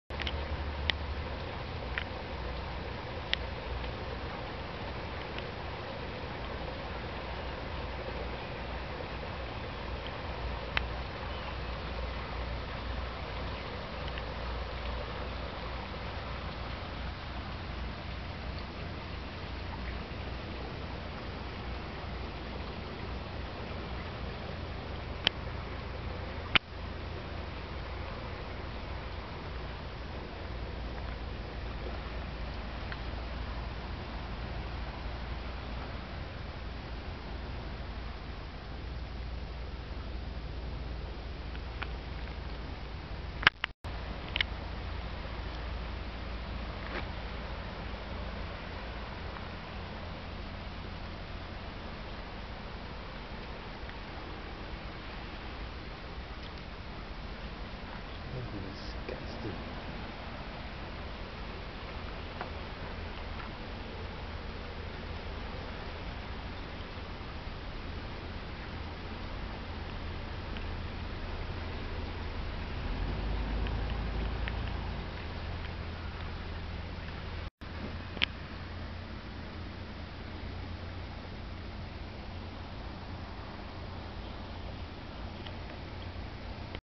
beach creepy crawlies
these little beach dwellin' guys look a lot like cockroaches, but have lots of legs.

Anyone know what they are?